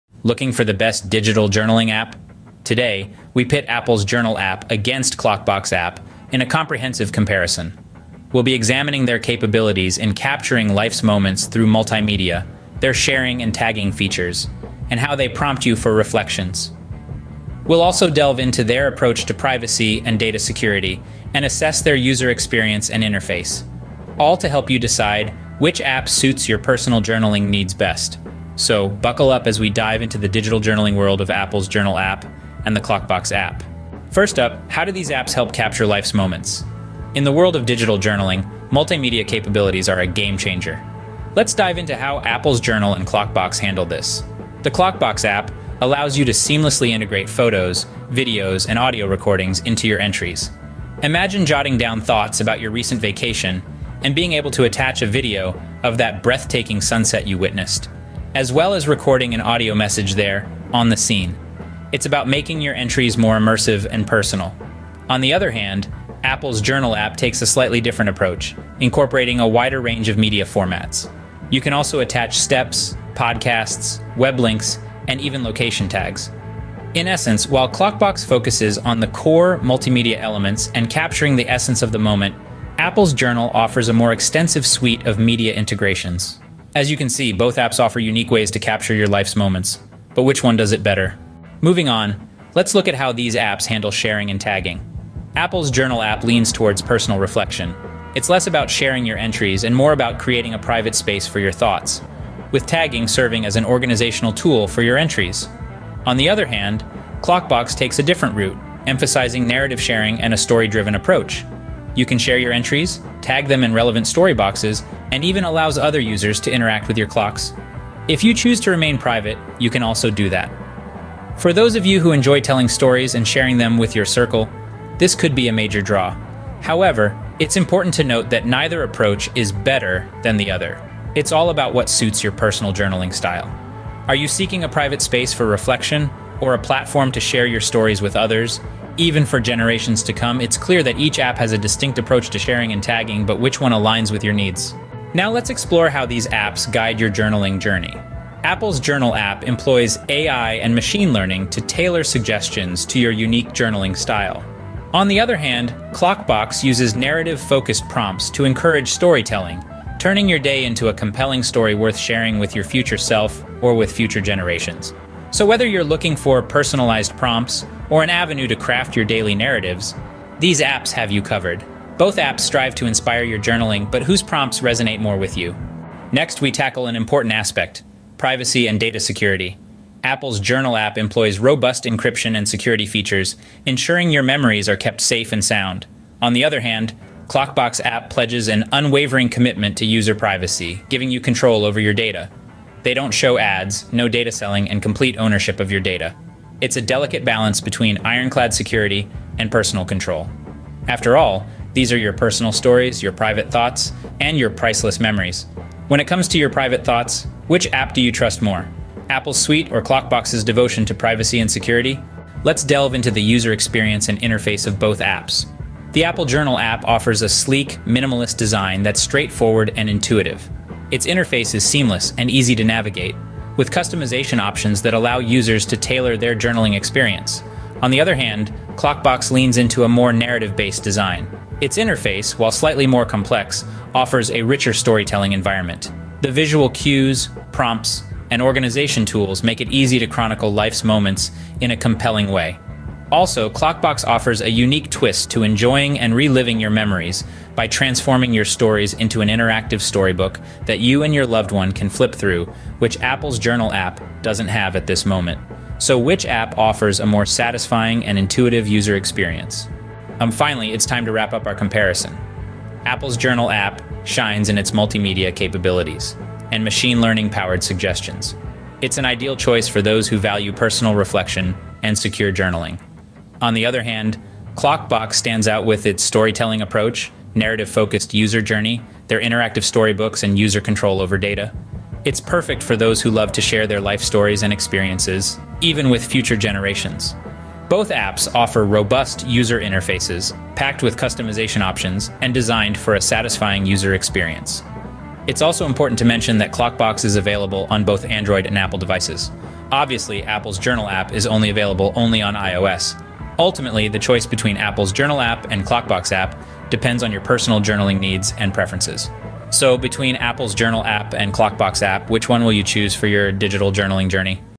Apple’s Journal App vs. Klokbox App: Which is the Best Digital Journaling App?
Join us as we embark on a detailed comparison of two leading digital journaling applications: Apple’s Journal App and Klokbox.

This in-depth analysis video aims to dissect the features, usability, and user experience of both apps, offering a clear perspective on how they cater to the modern-day journaler. In an era where capturing life’s moments digitally has become the norm, choosing the right platform to document your memories, thoughts, and experiences is crucial.

Apple’s Journal App, a part of the iOS ecosystem, brings the convenience of multimedia journaling with an intuitive interface and machine learning capabilities for a personalized experience. On the other hand, Klokbox focuses on narrative journaling, allowing users to share and preserve their stories with ease, emphasizing communal sharing and posthumous content release.

This video covers various aspects including the types of media supported by both apps, their sharing capabilities, AI and data privacy concerns, personalized suggestions, and how each app handles user data and privacy. Whether you're intrigued by the rich media integration and AI-driven prompts of Apple’s Journal App or the narrative-driven, privacy-centric approach of Klokbox, this comparison will help you make an informed decision.

Discover which app best suits your digital journaling needs, whether you prioritize a multimedia-rich experience or a platform for creating a lasting narrative legacy. Dive into our analysis to find out more!